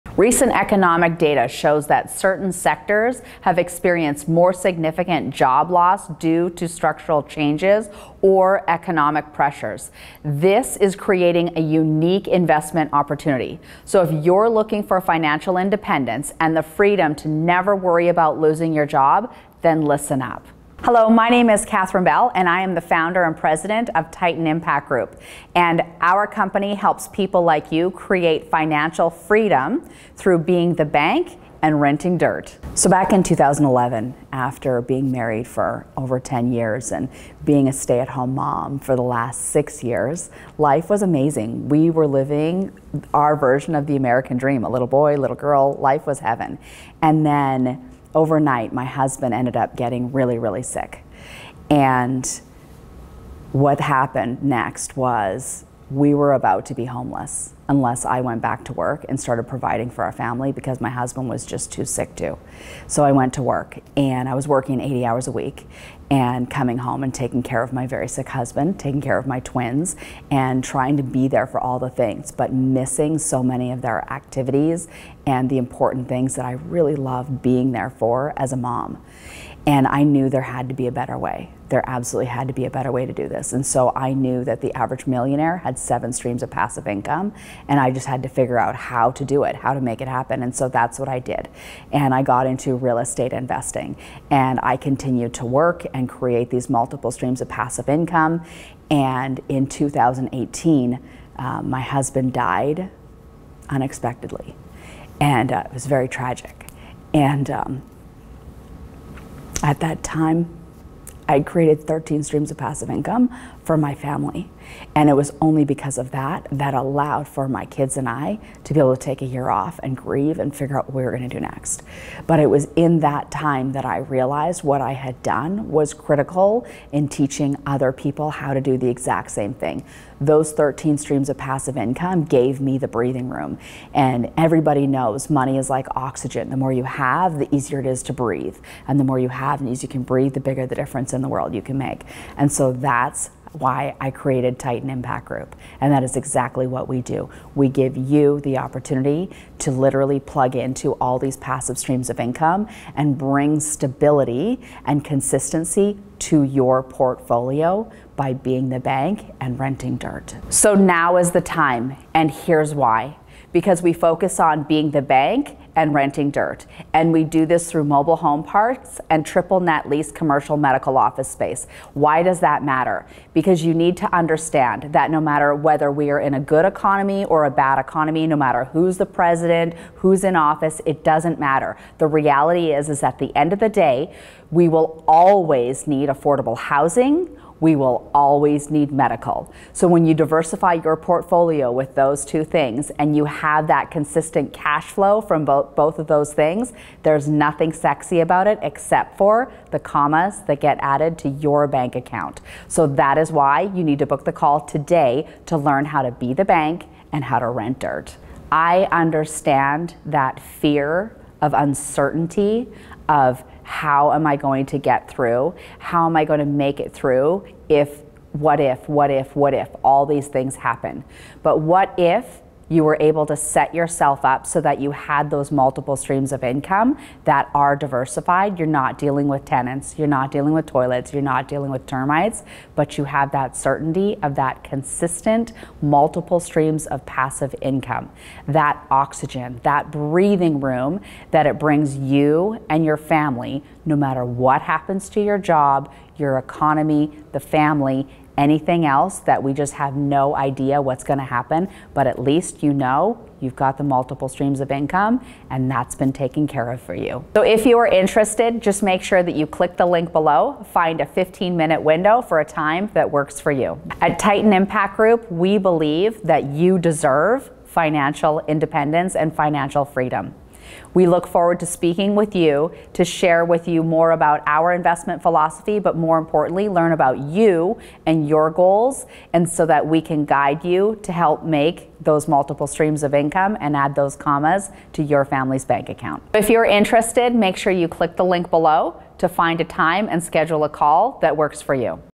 Overcoming Adversity: My Journey to Creating 13 Streams of Income | Titan Impact Group
Recent economic data shows that certain sectors have experienced more significant job loss due to structural changes or economic pressures. This is creating a unique investment opportunity. So, if you're looking for financial independence and the freedom to never worry about losing your job, then listen up.

Hello, my name is Catherine Bell, and I am the founder and president of Titan Impact Group. Our company helps people like you create financial freedom through being the bank and renting dirt.

So, back in 2011, after being married for over 10 years and being a stay-at-home mom for the last six years, life was amazing. We were living our version of the American dream: a little boy, a little girl—life was heaven. Then, overnight, my husband ended up getting really, really sick. What happened next was that we were about to be homeless unless I went back to work and started providing for our family because my husband was just too sick to do so.

I went to work, and I was working 80 hours a week, coming home and taking care of my very sick husband, taking care of my twins, and trying to be there for all the things, but missing so many of their activities and the important things that I really loved being there for as a mom. I knew there had to be a better way. There absolutely had to be a better way to do this.

I knew that the average millionaire had seven streams of passive income, and I just had to figure out how to do it—how to make it happen. And so that's what I did. I got into real estate investing, and I continued to work and create these multiple streams of passive income.

In 2018, my husband died unexpectedly. It was very tragic. At that time, I had created 13 streams of passive income for my family. It was only because of that, that it allowed my kids and me to take a year off to grieve and figure out what we were going to do next. It was during that time that I realized what I had done was critical in teaching other people how to do the exact same thing.

Those 13 streams of passive income gave me the breathing room—and everybody knows, money is like oxygen. The more you have, the easier it is to breathe. The more you have, the easier you can breathe, and the bigger the difference in the world you can make. And so that's why I created Titan Impact Group, and that is exactly what we do. We give you the opportunity to literally plug into all these passive streams of income and bring stability and consistency to your portfolio by being the bank and renting dirt.

So now is the time, and here's why: We focus on being the bank and renting dirt, and we do this through mobile home parks and triple net lease commercial medical office space. Why does that matter? Because you need to understand that no matter whether we are in a good economy or a bad economy, no matter who's the president or who's in office, it doesn't matter. The reality is, at the end of the day, we will always need affordable housing. We will always need medical facilities. So, when you diversify your portfolio with those two things and have that consistent cash flow from both, there's nothing sexy about it—except for the commas that get added to your bank account.

That is why you need to book the call today to learn how to be the bank and how to rent dirt. I understand that fear of uncertainty: How am I going to get through? How am I going to make it through? What if? What if? What if all these things happen? But what if you were able to set yourself up so that you had those multiple streams of income that are diversified? You're not dealing with tenants, you're not dealing with toilets, you're not dealing with termites, but you have that certainty of consistent multiple streams of passive income—that oxygen, that breathing room that it brings you and your family, no matter what happens to your job, your economy, your family, or anything else that we just have no idea what's going to happen. But at least you know you've got the multiple streams of income, and that's been taken care of for you.

So, if you are interested, just make sure that you click the link below, find a 15-minute window for a time that works for you. At Titan Impact Group, we believe that you deserve financial independence and financial freedom. We look forward to speaking with you to share more about our investment philosophy, but more importantly, to learn about you and your goals so that we can guide you to help make those multiple streams of income and add those commas to your family's bank account.